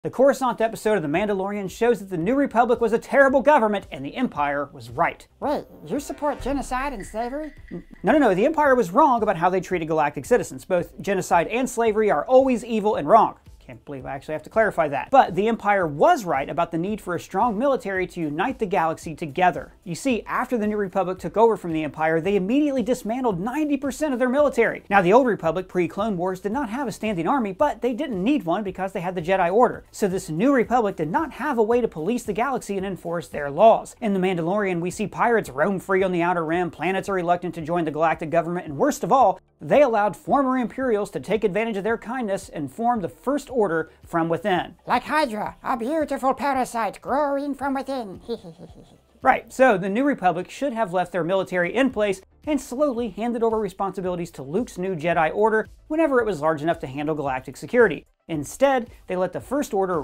The Mandalorian Proves the Empire Was Right
The Mandalorian Season 3 Episode 3 shows why the New Republic was weak, and Palpatine was better at galactic management